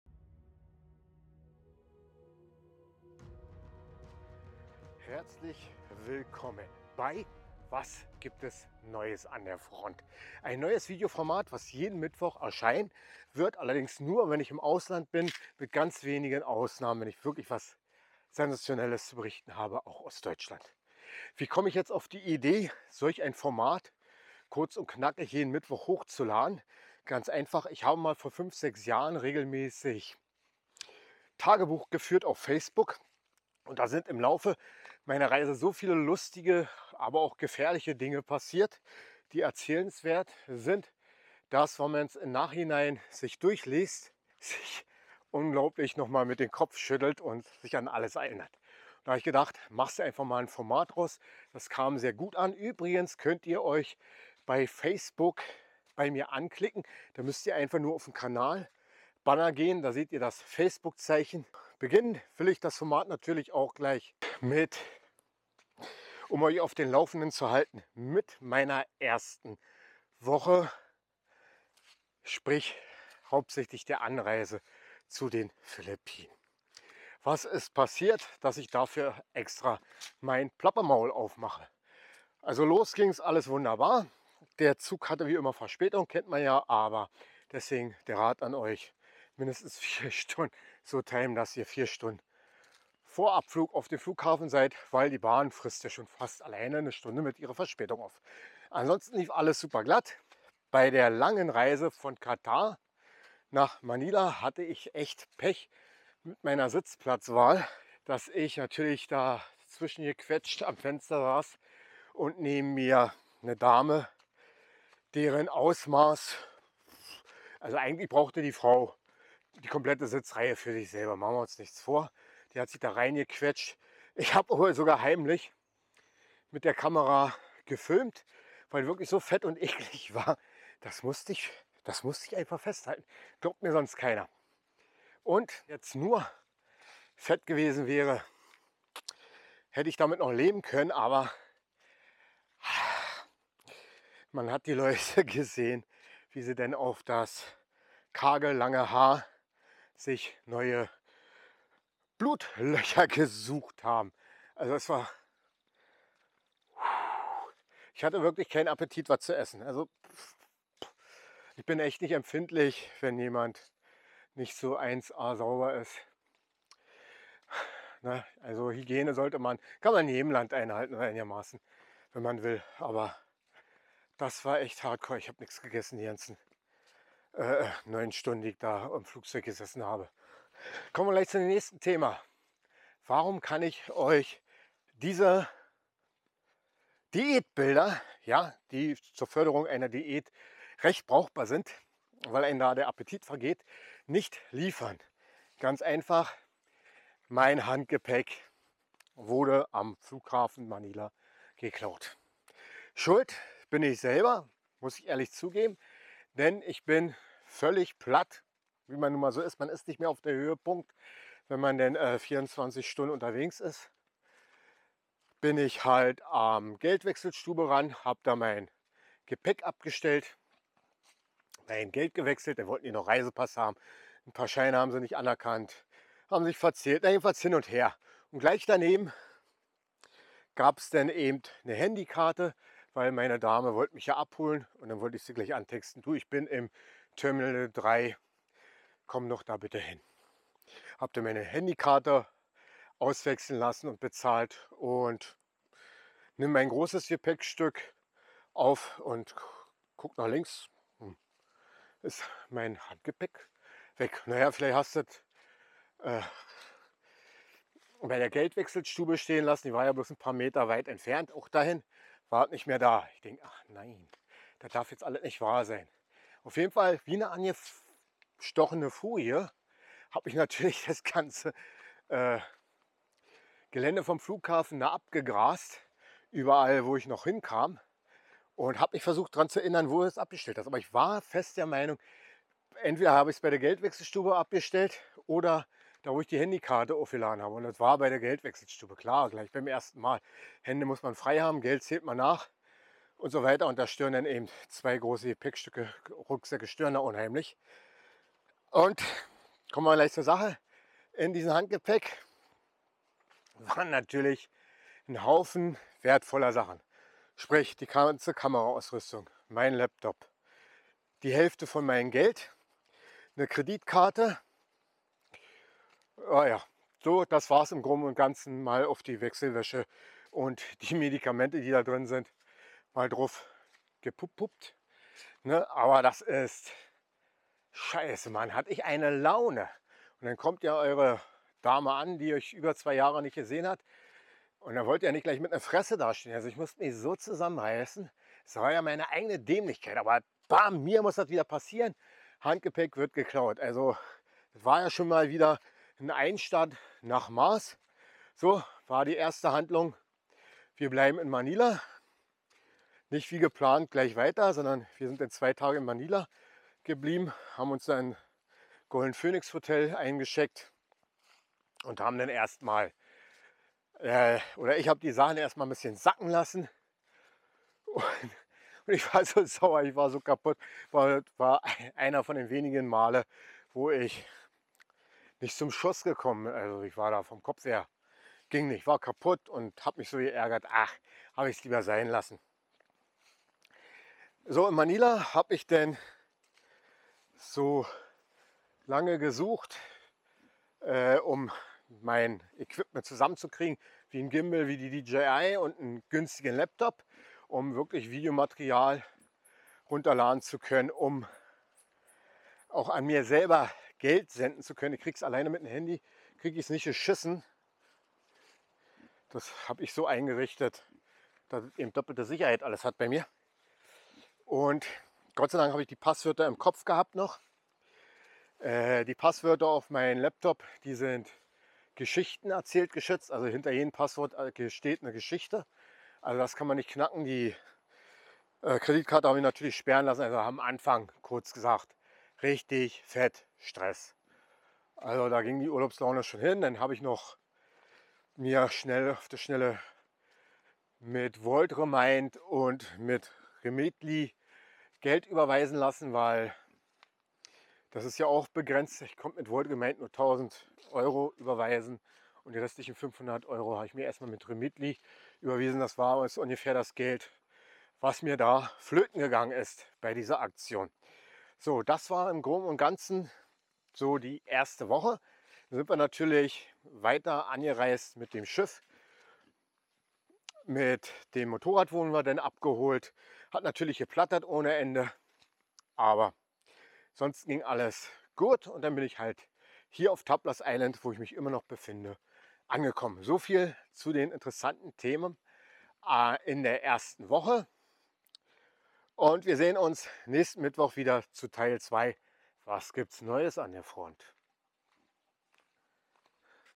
Was gibt es neues an der Front | Woche1 | Philippinen | 2022 | Asien | Info | meine Reise 🌴
In dieser Videoserie halte ich euch auf den laufenden!
Was alles in der letzten Woche passiert ist!